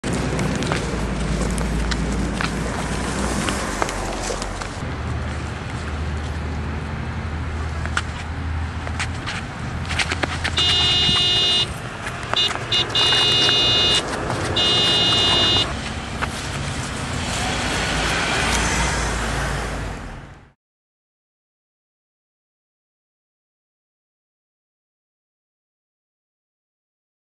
Frank Squirrel crossing the street
Like most squirrels, Frank has nightmares about crossing the street. The other day while crossing Lincoln, Frank's worst nightmare almost came true.